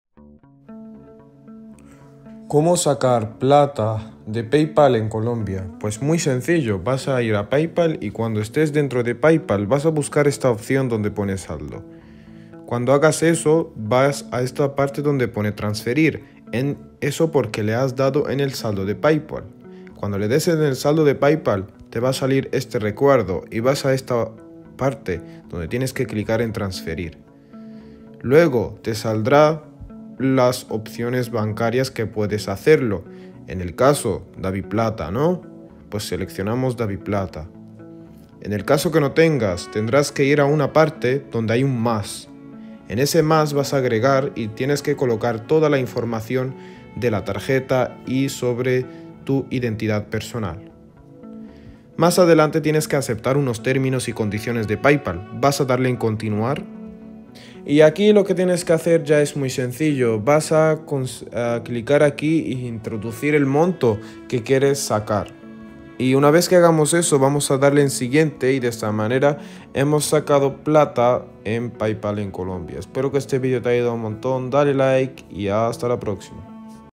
➡️Cómo SACAR PLATA En PayPal Desde COLOMBIA en 2024 (Paso a Paso)
Quieres sacar plata en PayPal en Colombia pero no sabes cómo hacerlo pues en este vídeo has encontrado la solución para conseguir hacerlo de manera sencilla y rápida